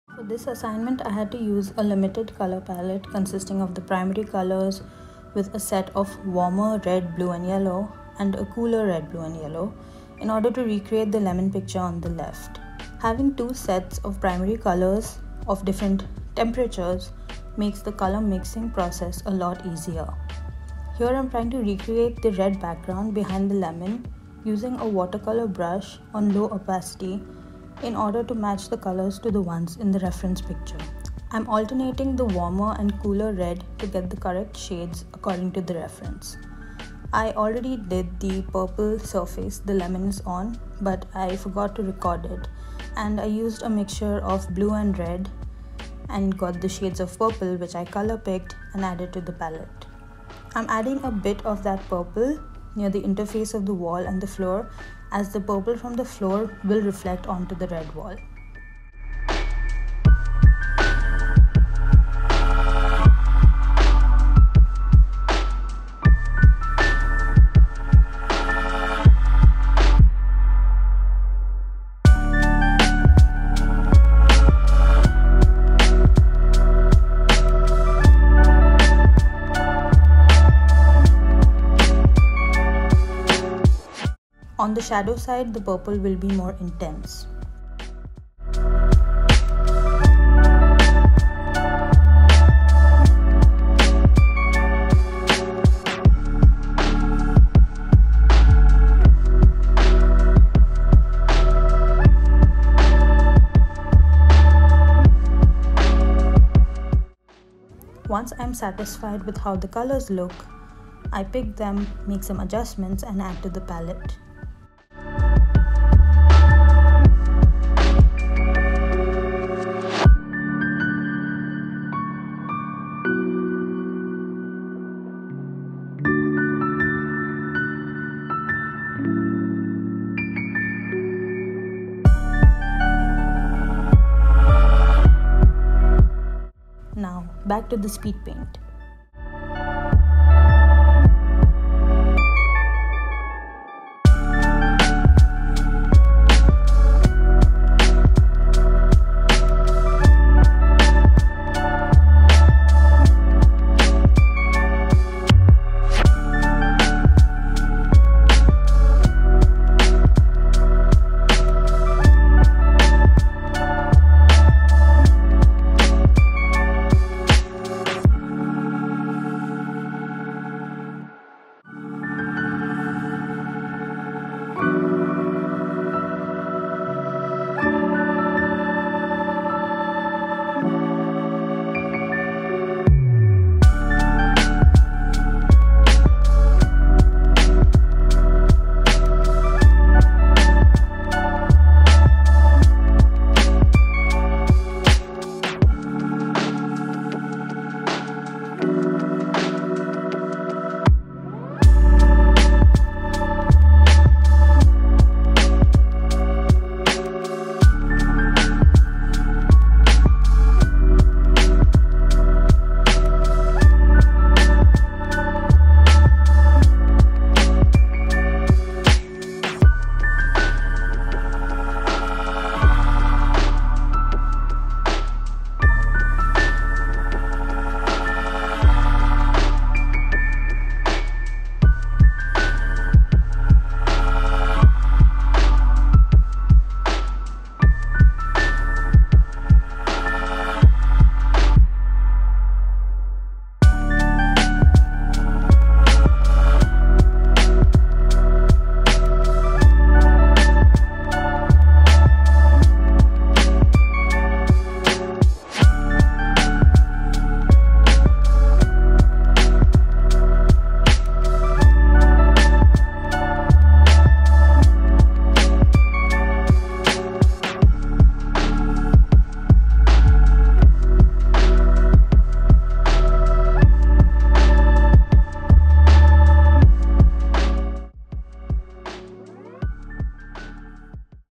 Colour Theory- Lemon Study (Speedpaint)
Another Skillshare assignment for JW Learning's colour theory skillshare class.